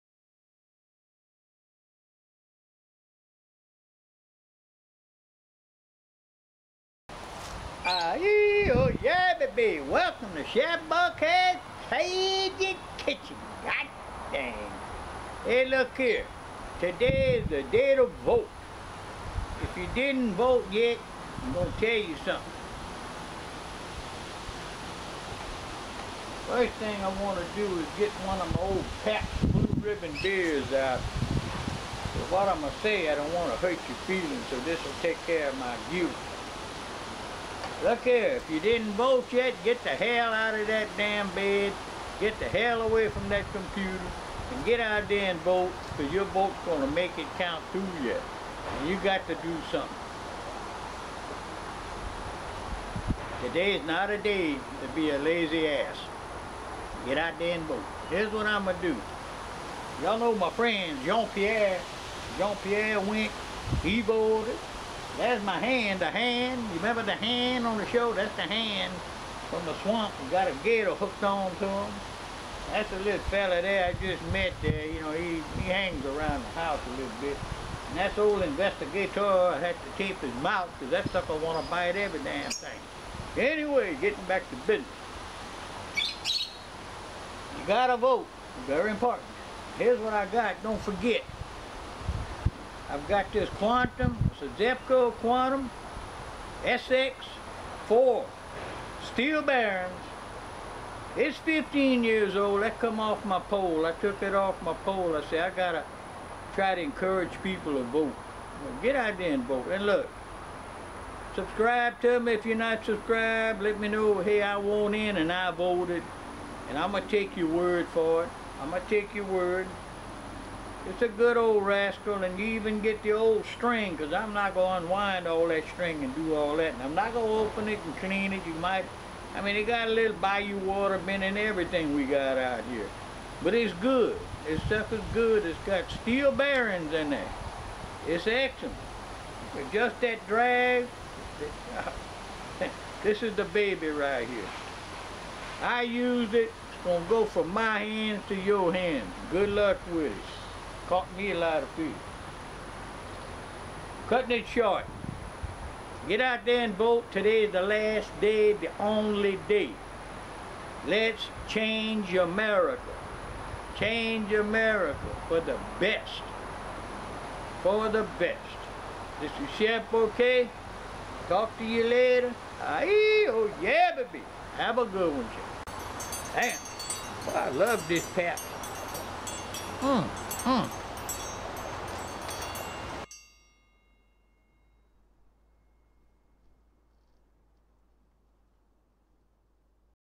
Get Out And Vote Day! Plus Reel Giveaway!
Chef Bourque' encourages all LEGAL voters to head to the polls and VOTE. This is Americas only chance to "Survive as a Nation."
Think of your kids and grandkids. Their tomorrow depends on your vote TODAY!